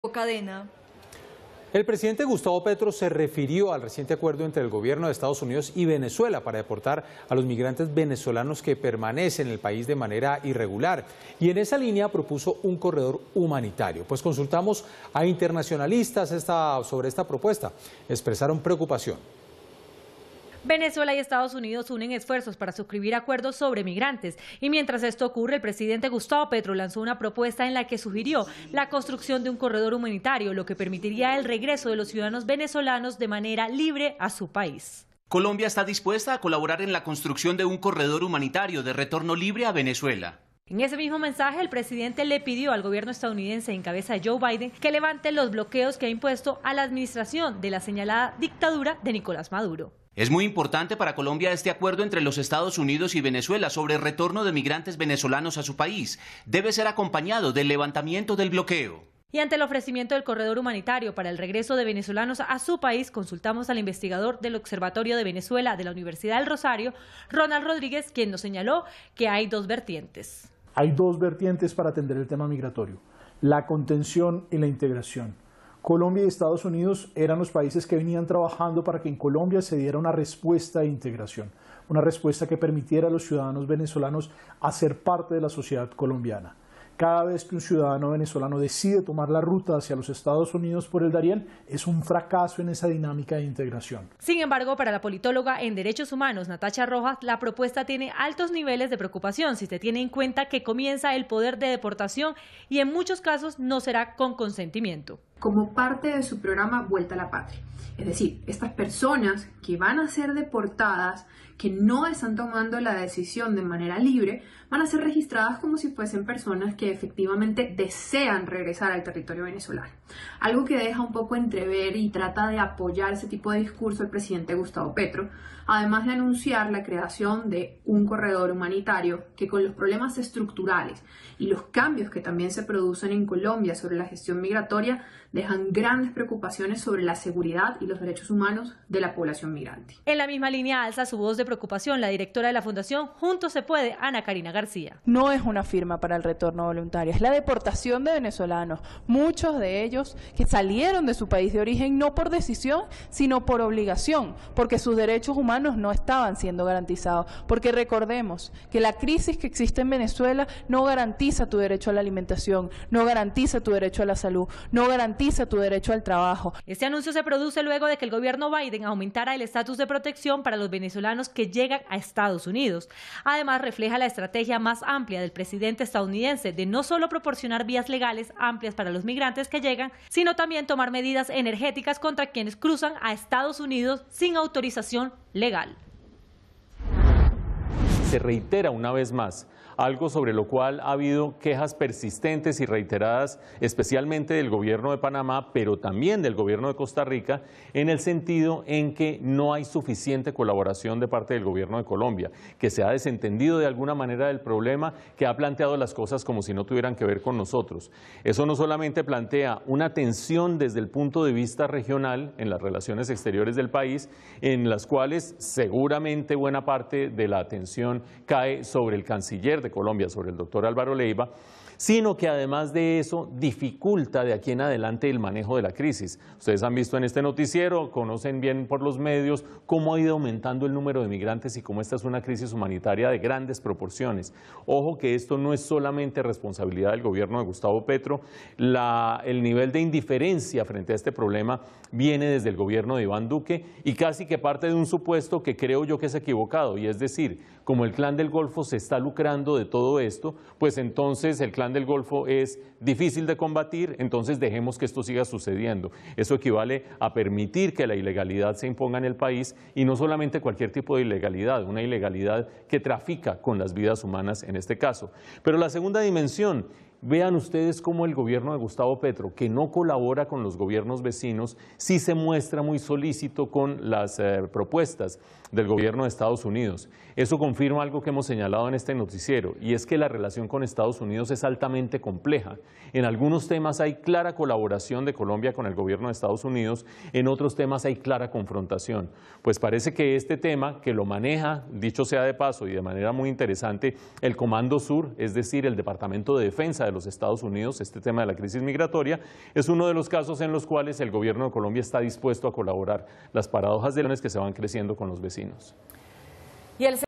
Expertos analizan la propuesta de crear un corredor humanitario entre Colombia y Venezuela
Ronal Rodríguez, investigador del Observatorio de Venezuela de la Universidad del Rosario, analiza para Red Más Noticias las implicaciones para los migrantes venezolanos de la propuesta del corredor humanitario hecha por Gustavo Petro.